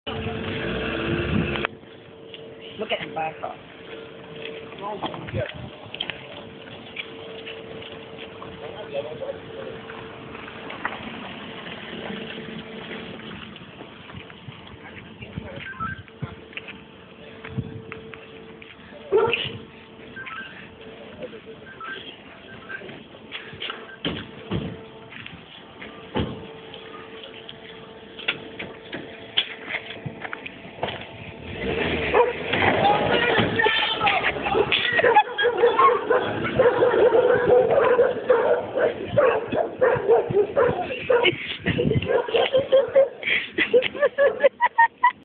Super ATRACO
Echo al mafioso se roban un autito de valle hermoso central